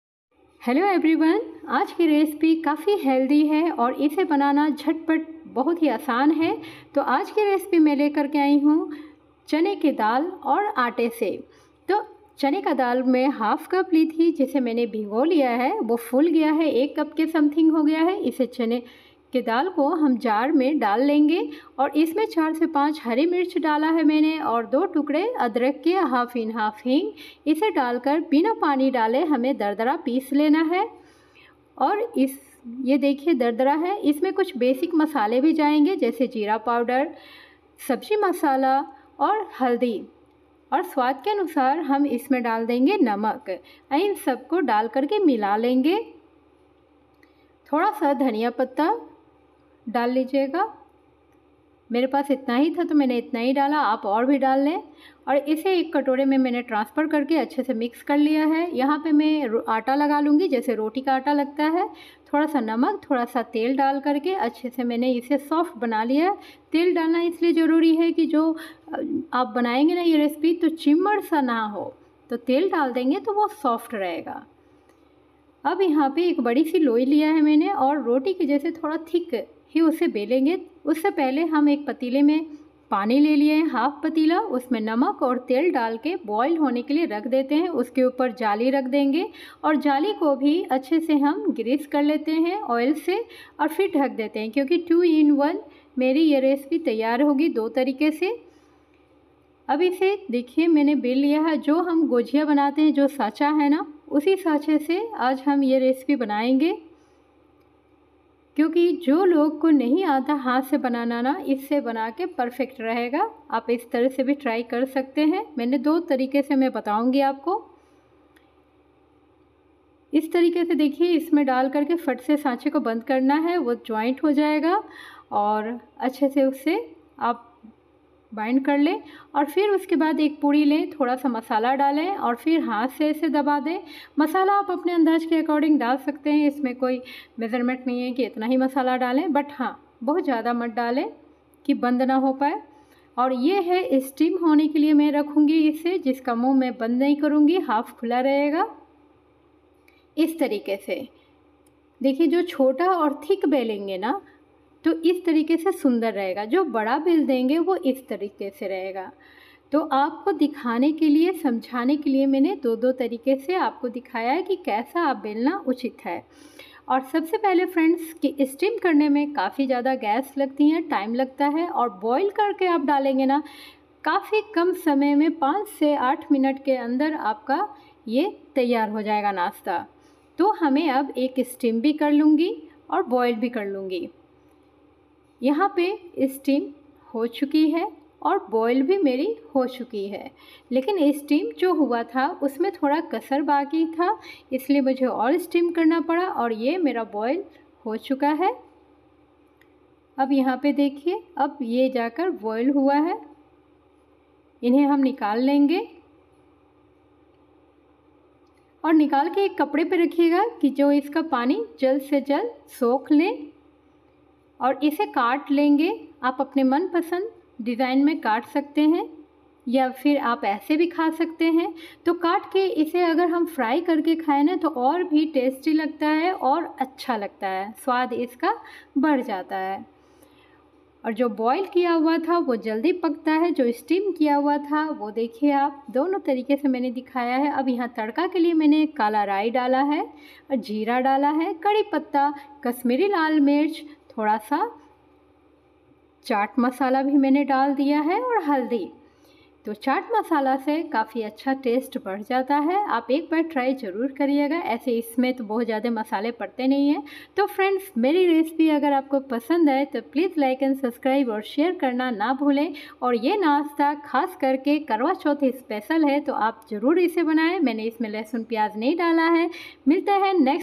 गेहूं के आटे और चने के दाल से बिना तेल का बनाएं आसान, स्वादिष्ट और सेहतमंद नाश्ता । Dal pitha 😋
गेहूं के आटे और चने के दाल से बिना तेल का बनाएं आसान, स्वादिष्ट और सेहतमंद नाश्ता । Dal pitha 😋 karva chauth special.